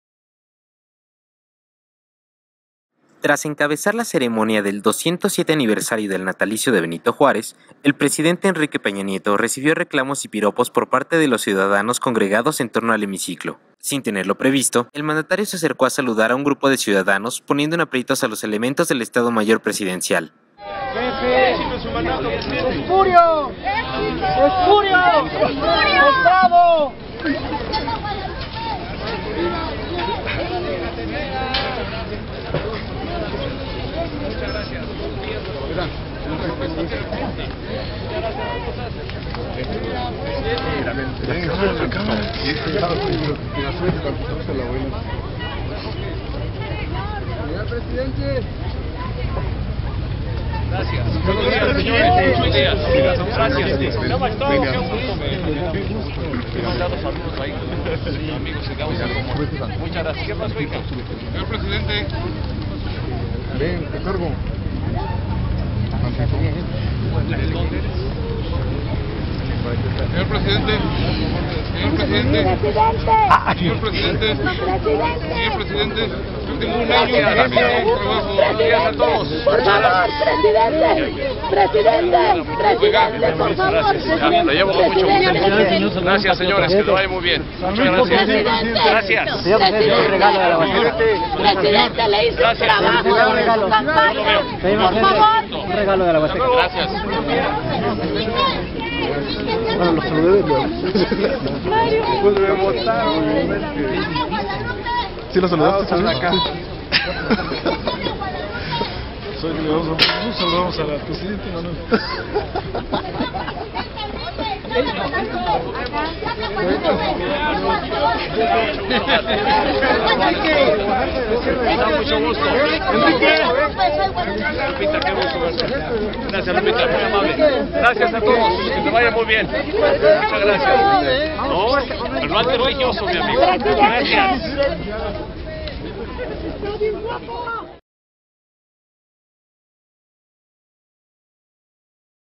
Halagan y reclaman a Peña en Hemiciclo
Tras encabezar la ceremonia del 207 aniversario del Natalicio de Benito Juárez, el Presidente Enrique Peña Nieto recibió reclamos y piropos por parte de los ciudadanos congregados en torno al Hemiciclo a Juárez.